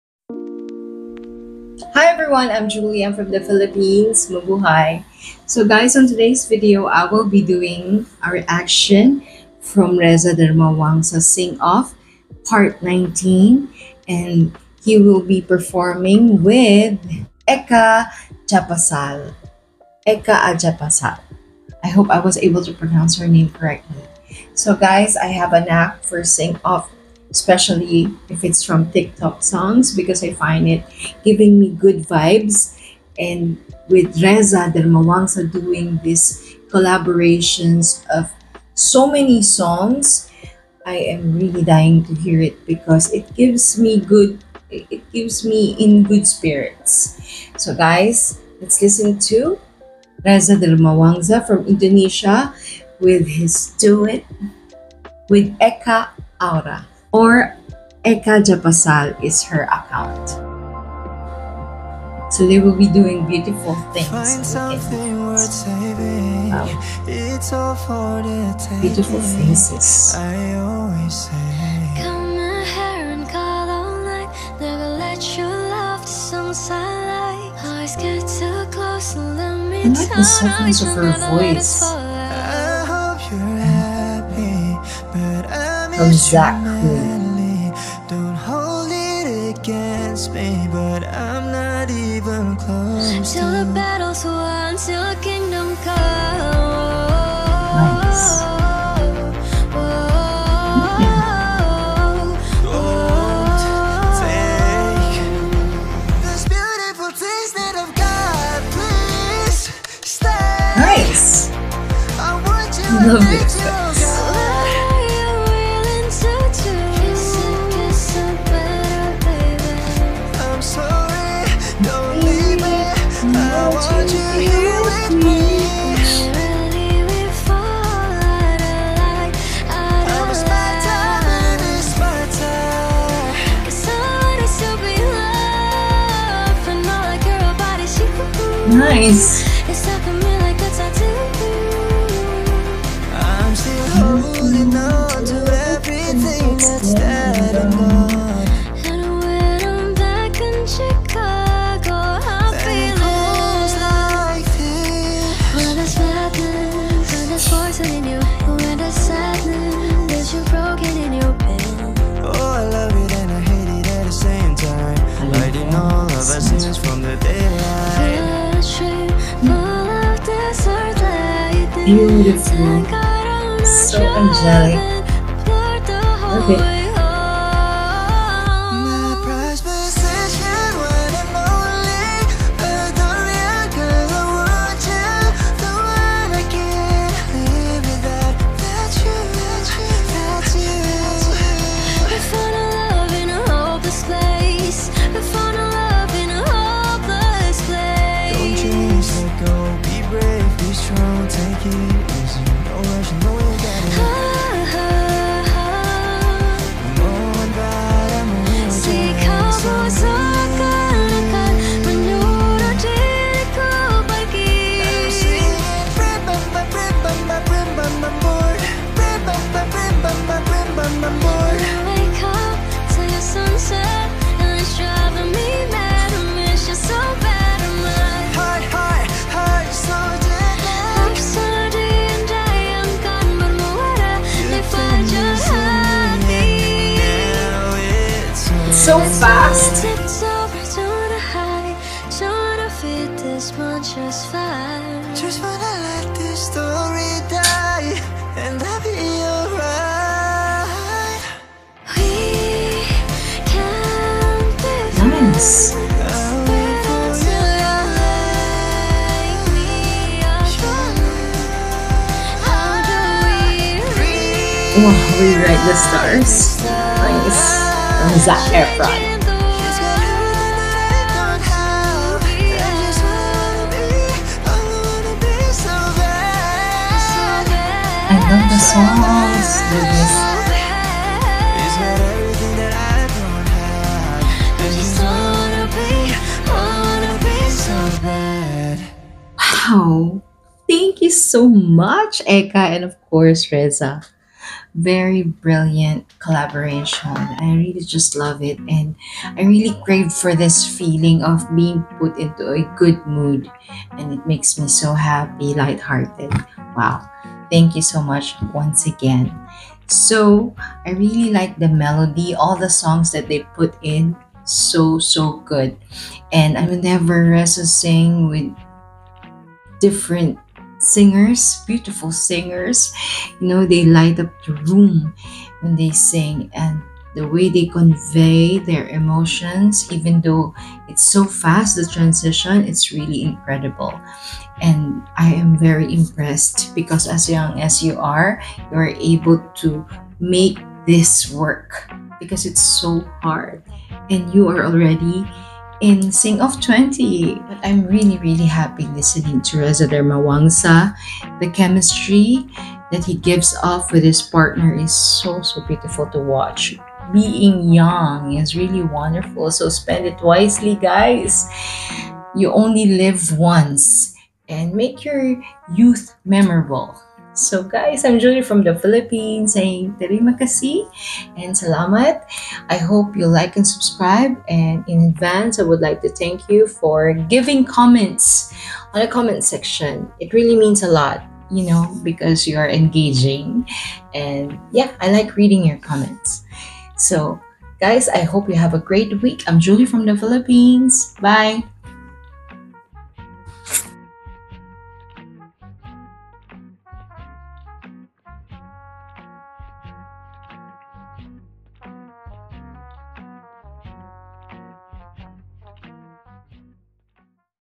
REZA DARMAWANGSA and ECA AURA |'SING-OFF 19' | TIKTOK SONGS| Love their chemistry 😍
This is Reza Darmawangsa  from Indonesia with Eca Aura  and they collaborated for 'SINGOFF TIKTOK SONGS' PART 19 . I hope you will  be entertained and enjoy it.

THANKS